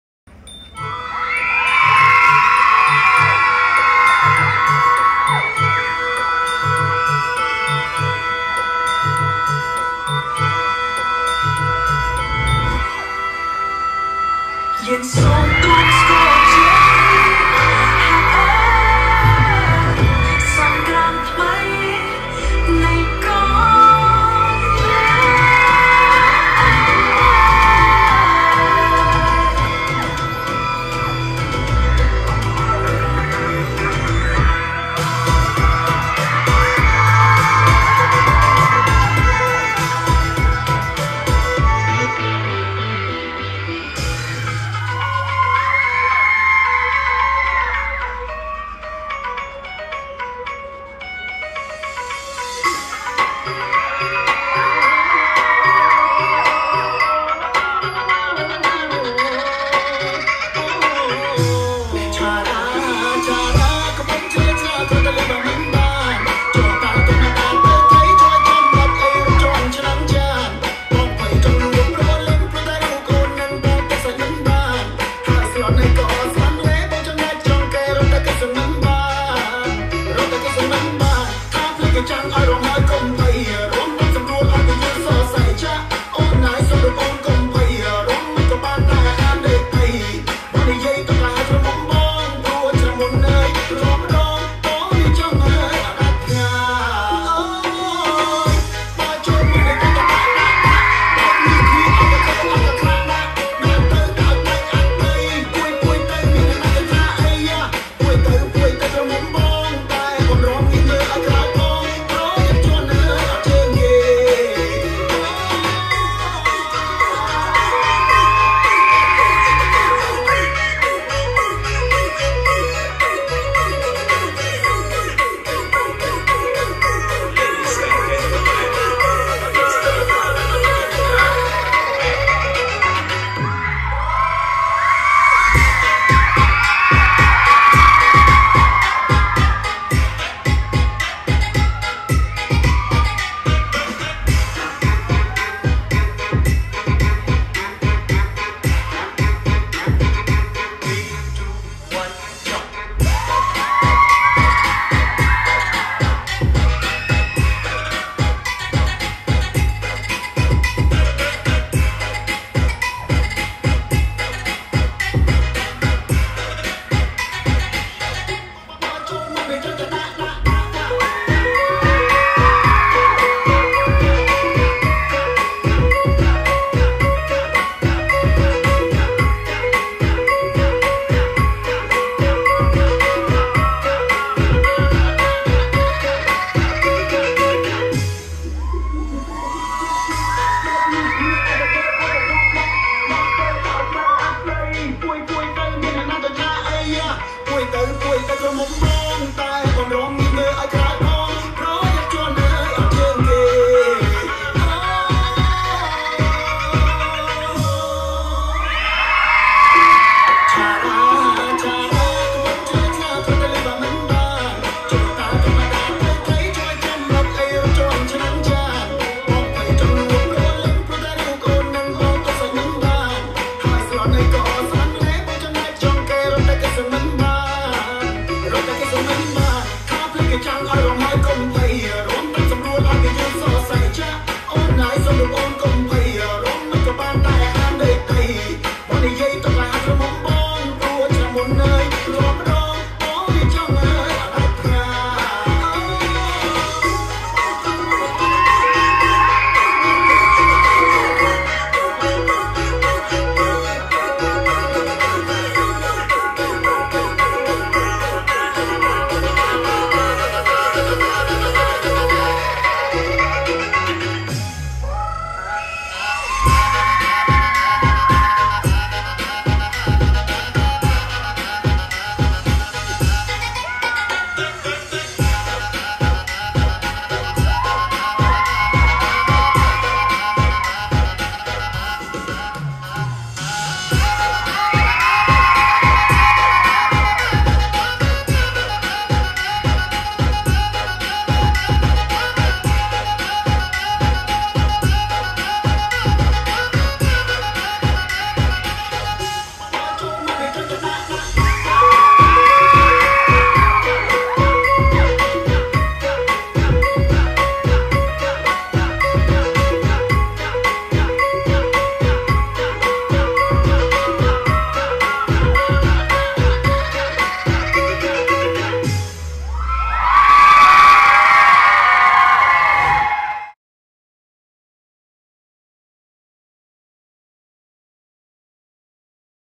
3HMONGTV | Viv Ncaus Koom Siab | Dance Competition | Oklahoma Hmong New Year 2023.
We're bringing news to you as it happens in Minnesota's Hmong community and around the country.